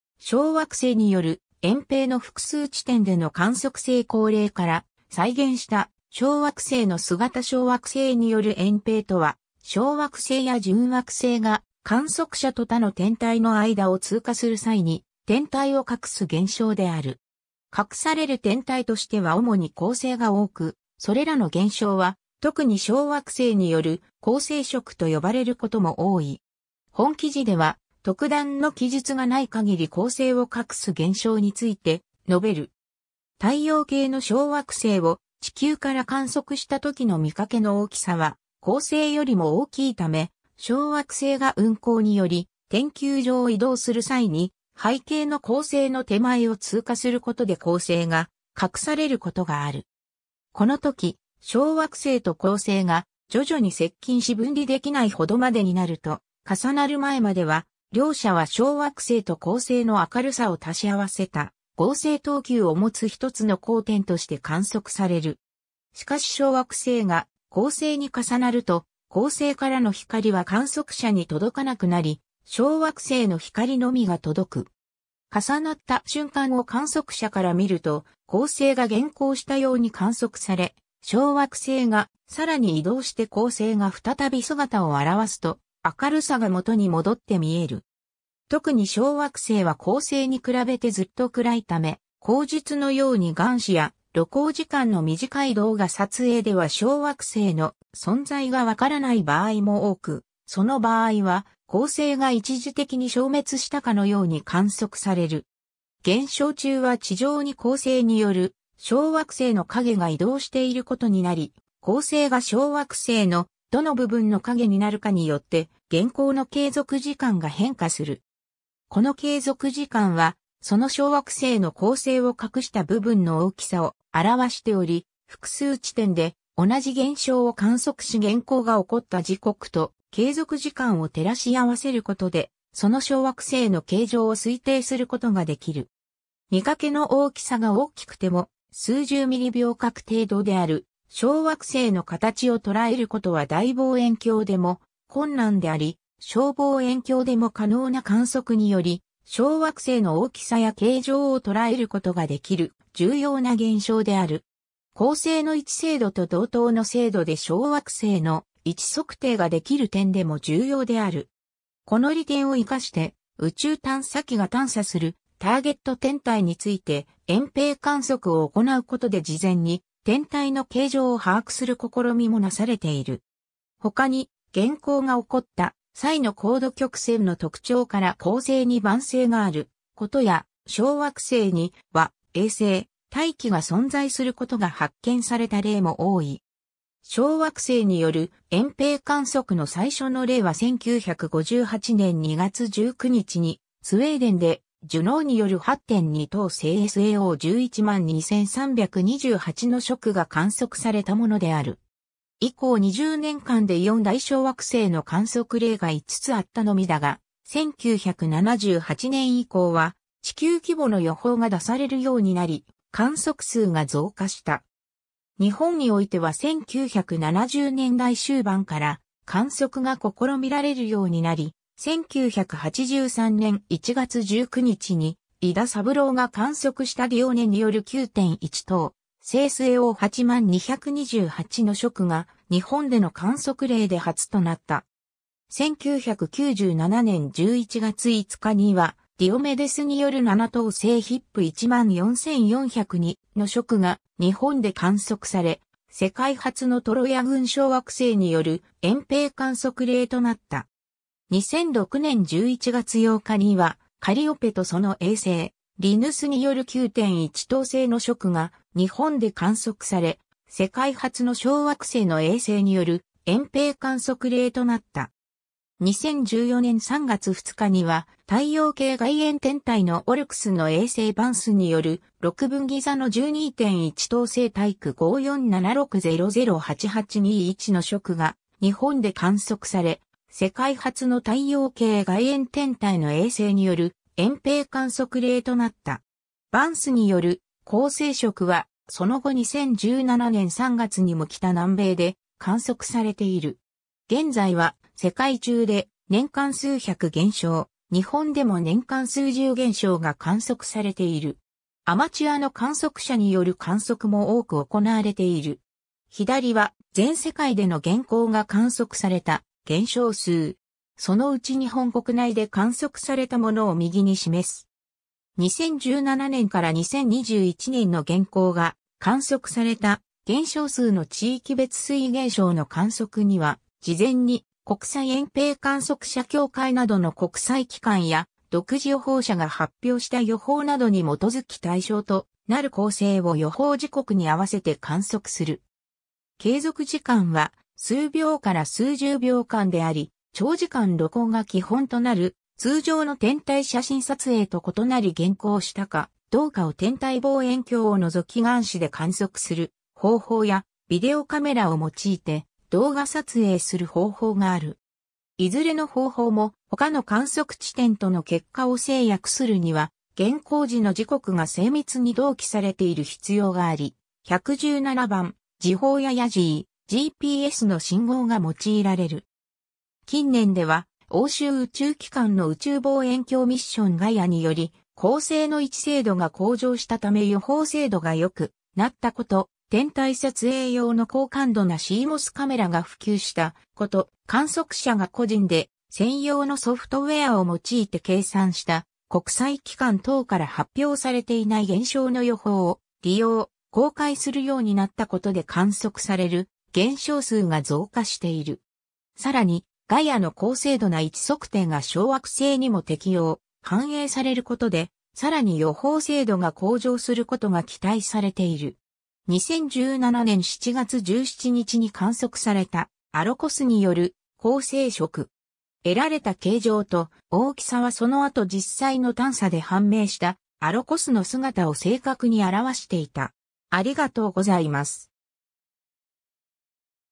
小惑星による掩蔽
食_(天文)
小惑星による掩蔽の複数地点での観測成功例から再現した小惑星の姿 小惑星による掩蔽（しょうわくせいによるえんぺい、asteroid occultation）とは、小惑星や準惑星が観測者と他の天体の間を通過する際に、天体を隠す現象である。
隠される天体としては主に恒星が多く、それらの現象は特に小惑星による恒星食と呼ばれることも多い。
本記事では特段の記述がない限り恒星を隠す現象について述べる。
 太陽系の小惑星を地球から観測したときの見かけの大きさは恒星よりも大きいため、小惑星が運行により天球上を移動する際に背景の恒星の手前を通過することで恒星が隠されることがある。
このとき、小惑星と恒星が徐々に接近し分離できないほどまでになると、重なる前までは両者は小惑星と恒星の明るさを足し合わせた合成等級を持つ1つの光点として観測される。
しかし小惑星が恒星に重なると、恒星からの光は観測者に届かなくなり小惑星の光のみが届く。
重なった瞬間を観測者から見ると、恒星が減光したように観測され、小惑星がさらに移動して恒星が再び姿を現すと明るさが元に戻って見える。
特に小惑星は恒星に比べてずっと暗いため後述のように眼視や露光時間の短い動画撮影では小惑星の存在が分からない場合も多く、その場合は恒星が一時的に消滅したかのように観測される。
 現象中は地上に恒星による小惑星の影が移動していることになり、恒星が小惑星のどの部分の影になるかによって減光の継続時間が変化する。
この継続時間はその小惑星の恒星を隠した部分の大きさ(弦の長さ)を表しており、複数地点で同じ現象を観測し減光が起こった時刻と継続時間を照らし合わせることでその小惑星の形状を推定することができる。
 見かけの大きさが大きくても数十ミリ秒角程度である小惑星の形を捉えることは大望遠鏡でも困難であり、小望遠鏡でも可能な観測により小惑星の大きさや形状を捉えることができる重要な現象である。
恒星の位置精度と同等の精度で小惑星の位置測定ができる点でも重要である。
 この利点を生かして宇宙探査機が探査するターゲット天体について掩蔽観測を行うことで事前に天体の形状を把握する試みもなされている。
 ほかに、減光が起こった際の光度曲線の特徴から恒星に伴星があることや、小惑星に輪・衛星・大気が存在することが発見された例も多い。
 小惑星による掩蔽観測の最初の例は1958年2月19日にスウェーデンで（3）ジュノーによる8.2等星SAO112328の食が観測されたものである。
以降20年間で4大小惑星の観測例が5つあったのみだが、1978年以降は地球規模の予報が出されるようになり観測数が増加した。
 日本においては1970年代終盤から観測が試みられるようになり、1983年1月19日に井田三良が観測した(106) ディオネによる9.1等星SAO80228の食が日本での観測例で初となった。
 1997年11月5日には（1437）ディオメデスによる7等星HIP14402の食が日本で観測され、世界初のトロヤ群小惑星による掩蔽観測例となった。
 2006年11月8日には（22）カリオペとその衛星リヌスによる9.1等星の食が日本で観測され、世界初の小惑星の衛星による掩蔽観測例となった。
 2014年3月2日には太陽系外縁天体の（90482）オルクスの衛星ヴァンスによるろくぶんぎ座の12.1等星TYC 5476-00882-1の食が日本で観測され、世界初の太陽系外縁天体の衛星による掩蔽観測例となった。
ヴァンスによる恒星食はその後2017年3月にも北南米で観測されている。
 現在は世界中で年間数百現象、日本でも年間数十現象が観測されている。
アマチュアの観測者による観測も多く行われている。
 左は全世界での減光が観測された現象数(年別,2021年6月30日時点)。
そのうち日本国内で観測されたもの(年別,2021年8月16日時点)を右に示す。
 2017年から2021年の減光が観測された現象数の地域別推移(年別、2021年6月30日時点) 現象の観測には、事前に国際掩蔽観測者協会 (IOTA)などの国際機関や独自予報者が発表した予報などに基づき対象となる恒星を予報時刻に合わせて観測する。
継続時間は数秒から数十秒間であり、長時間露光が基本となる通常の天体写真撮影と異なり減光したかどうかを天体望遠鏡をのぞき眼視で観測する方法や、ビデオカメラを用いて動画撮影する方法がある。
 いずれの方法も、他の観測地点との結果を整約するには...